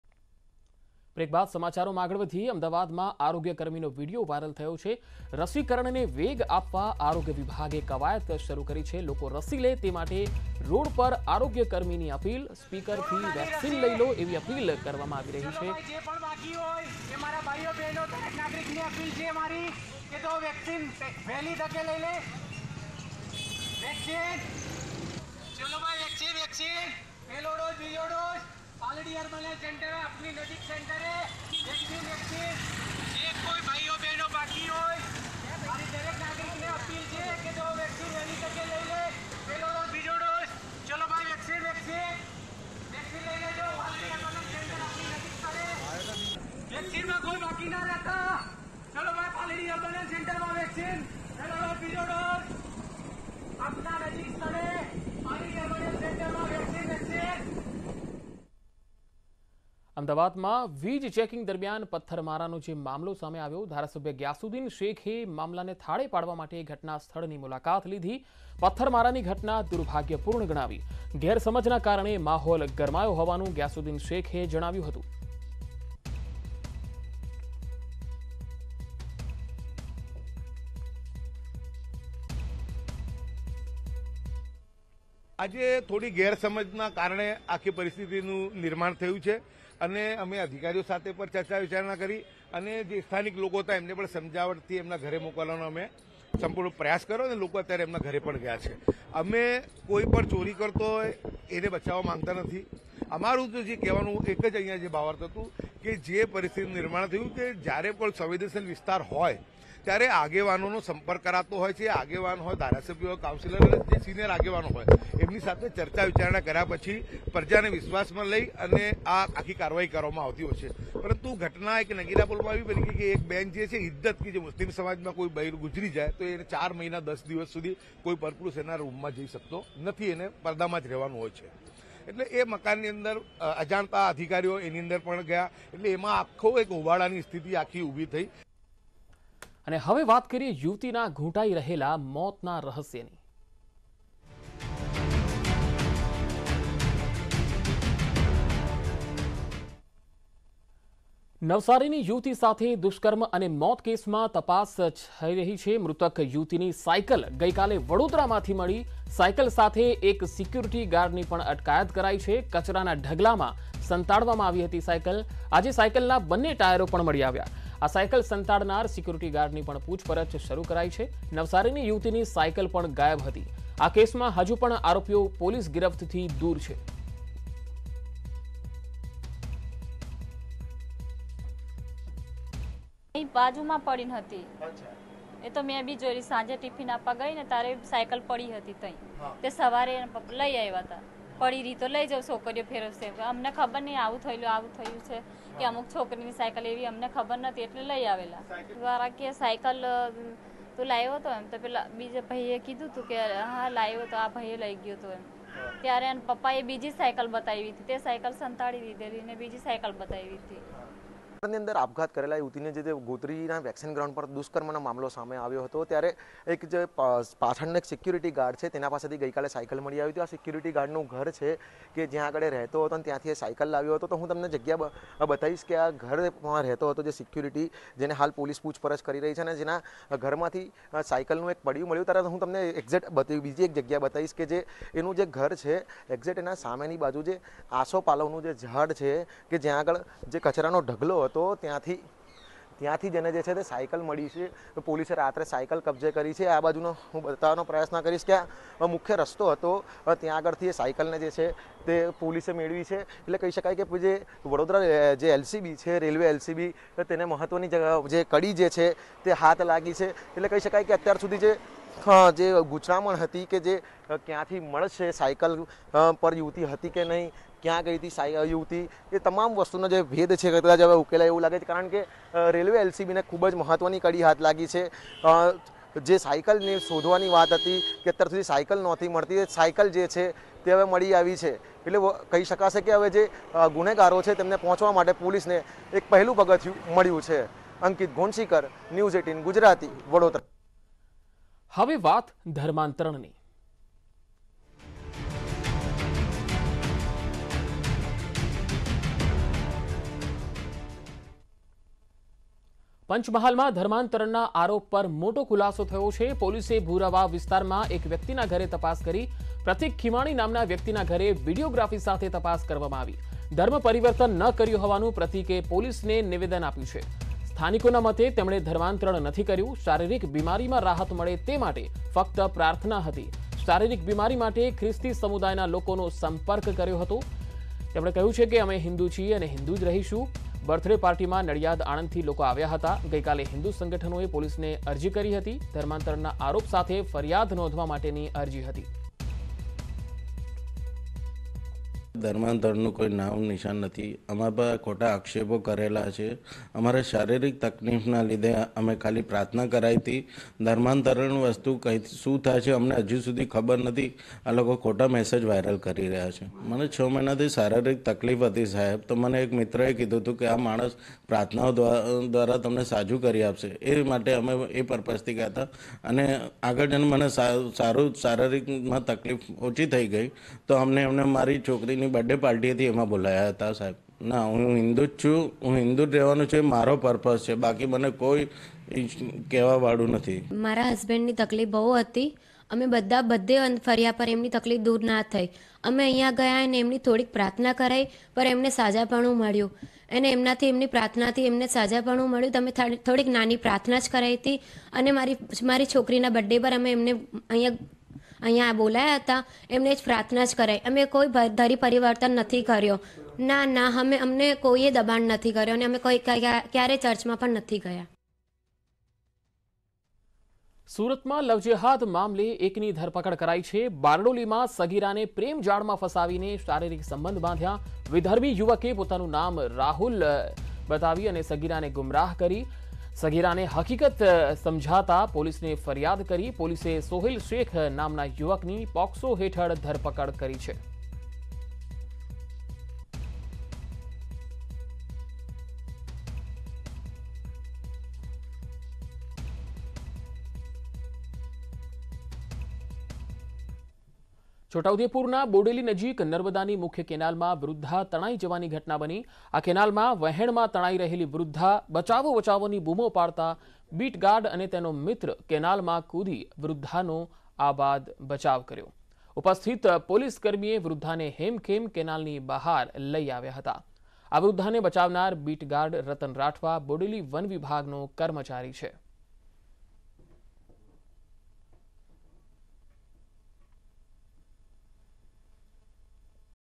"પહેલો ડોઝ, બીજો ડોઝ બાકી હોય તેઓ રસી લઇ લે'' ના નારા સાથે આરોગ્યકર્મી રસ્તા પર | News18 Gujarati
Ahmedabad માં આરોગ્યકર્મીનો Video Viral. રસીકરણને વેગ આપવા આરોગ્ય વિભાગની કવાયત. લોકોને રસી લેવા માટે Speaker થી Vaccine લઇ લો... એવી રોડ પર અપીલ કરતા જોવા મળ્યા.